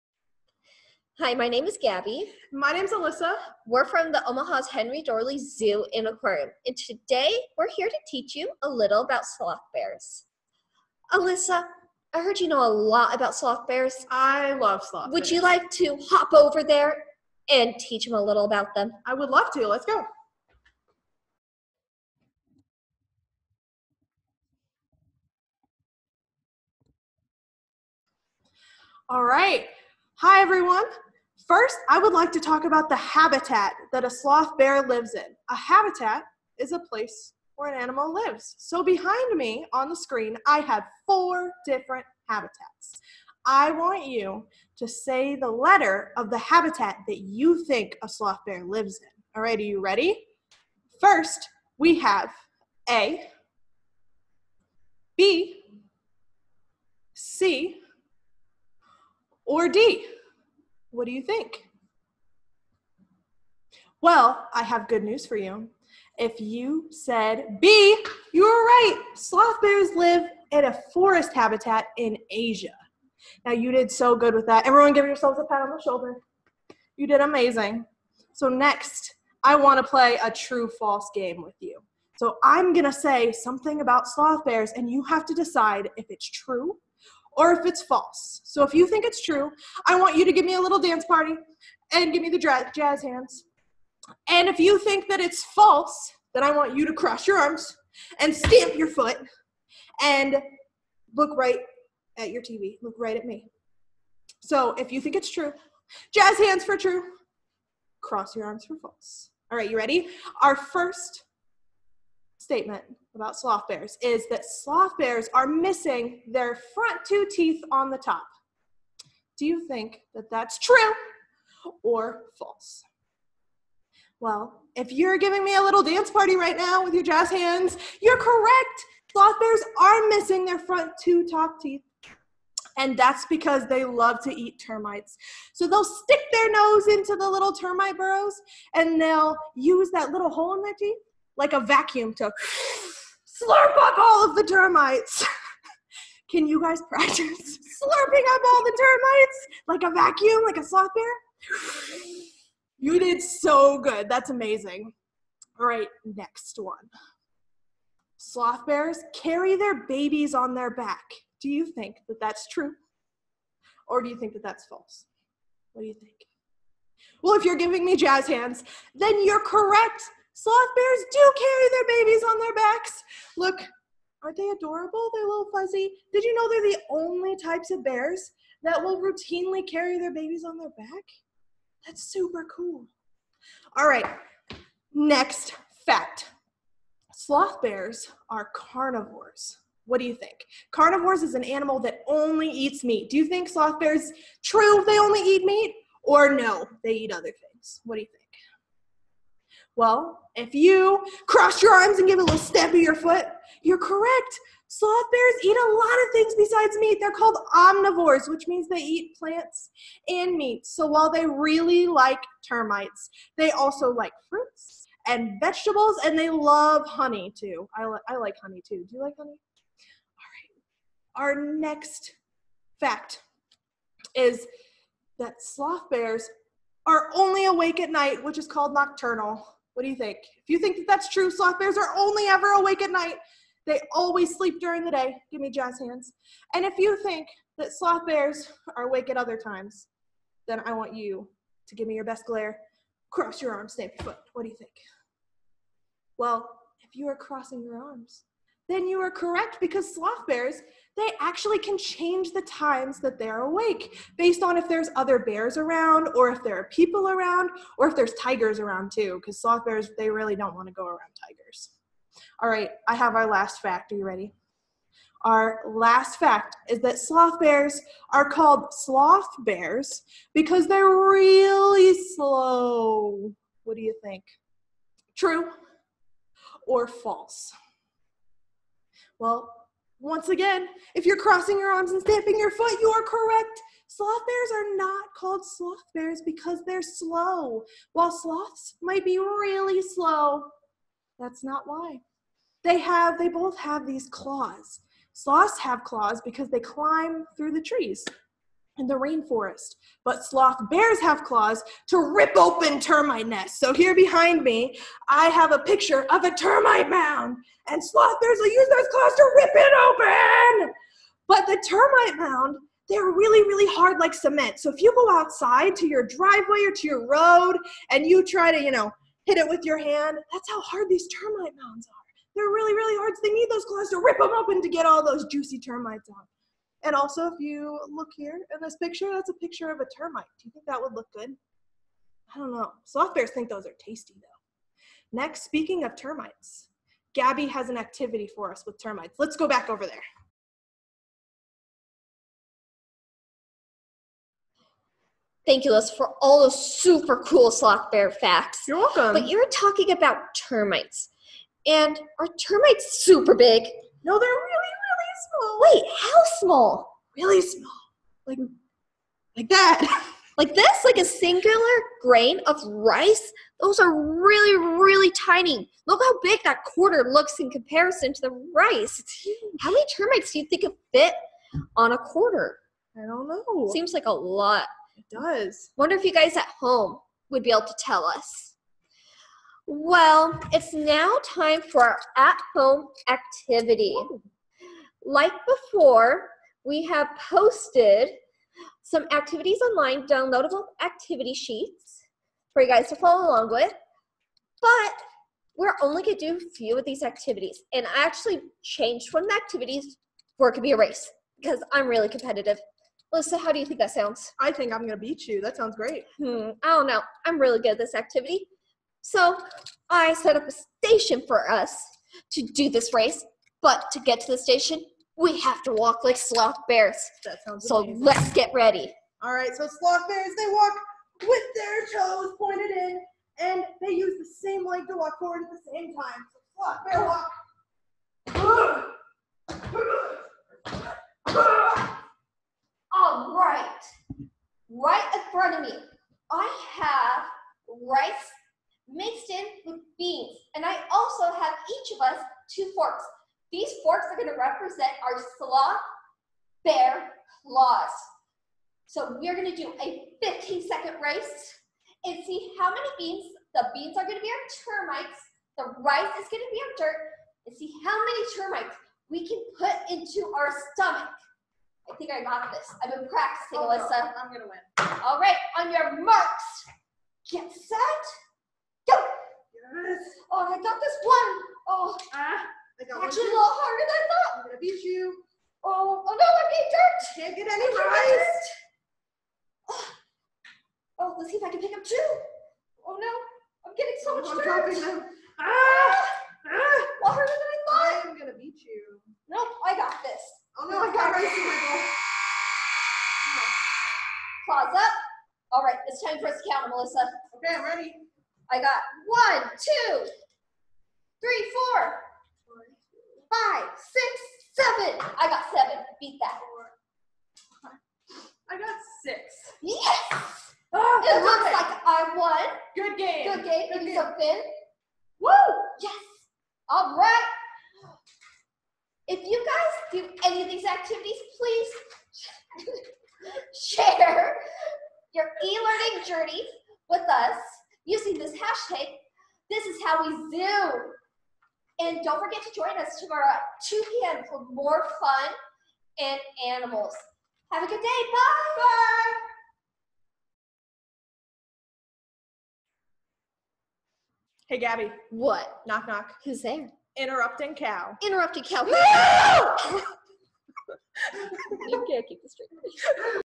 Zoo From Home :: Episode 2 - Bodo the Sloth Bear at Omaha's Henry Doorly Zoo and Aquarium
Sloth bears are one of eight species of bear and have no relation to sloths. Not only are they the only bear to routinely carry their cubs on their back, they have a fantastic ability to find insects. Sloth bears can close their nostrils tightly as a barrier from the insects they suck up. This “barrier” works for liquids too. Our animal care staff uses straws with Bodo, our 11-year-old male, to help distribute liquid medicine if needed. This medicine is mixed with coconut water or watered down juice to make it more desirable for the bears. Bodo even uses a reusable straw, which means so can you!